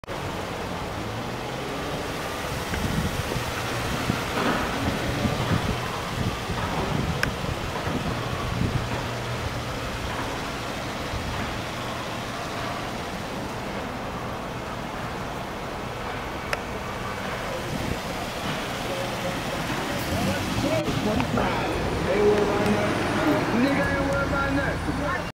2014 Jeep Wrangler Unlimited Rubicon | Granite Crystal | EL235317 | Seattle | Bellevue
Wrangler Unlimited Rubicon, 4D Sport Utility, 3.6L V6 24V VVT, 6-Speed Manual, 4WD, Granite, ** SATELLITE RADIO**, **ALLOY WHEELS**, **BLUETOOTH**, **HEATED SEATS**, and **LEATHER**.